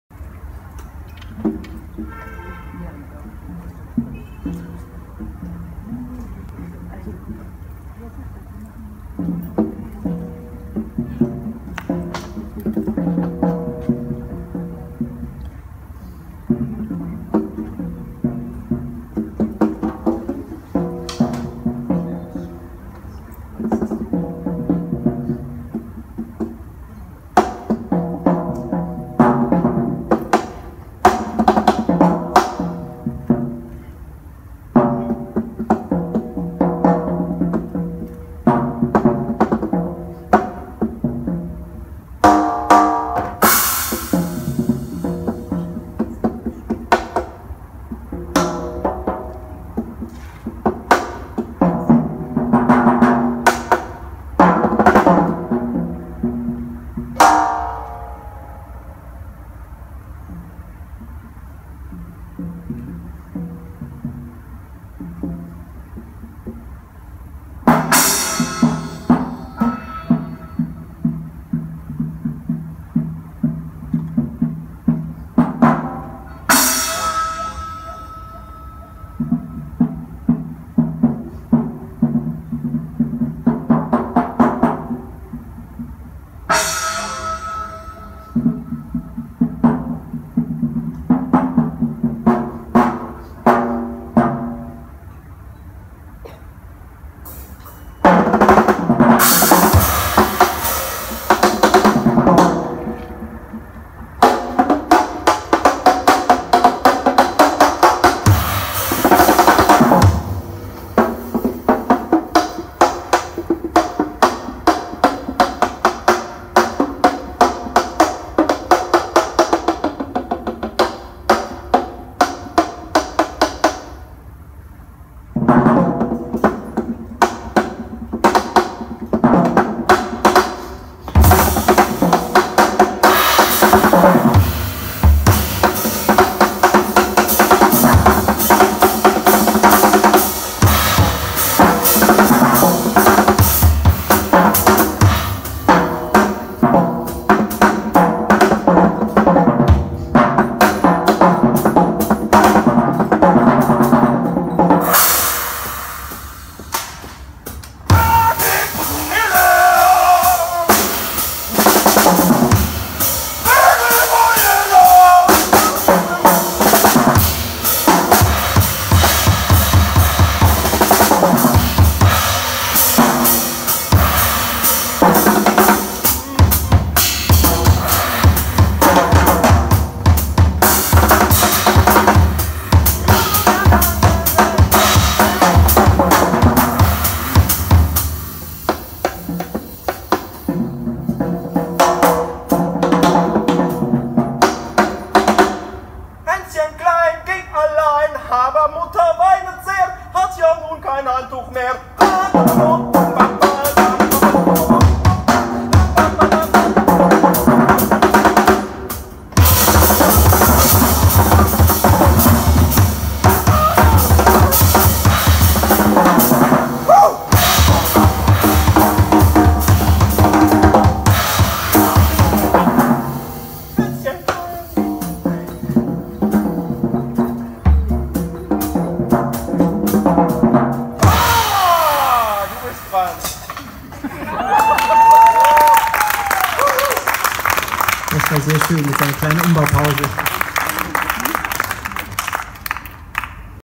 lutz jahnke drum solo +++LIVE+++ @ parkside studios
konzert @ parkside studios offenbach

Der Ausnahmecellist Christopher Herrmann erschafft seit seinem 14. Lebensjahr auf barockem, modernem und elektrischem Cello unterschiedlichste, musikalische Landschaften. Neben Klassik und seinem Interesse an deren Moderne hat ihn die Neugier auf Musik anderer Kulturen und des Jazz dazu inspiriert, seine eigene Klangsprache zu entwickeln.

Lutz Jahnke spielt seit 1991 auf allem herum was klang erzeugt und hat jahre später erst in frankfurt gerlernt, was man tun muss, damit andere musiker endlich spaß beim zusammenspiel haben — lol.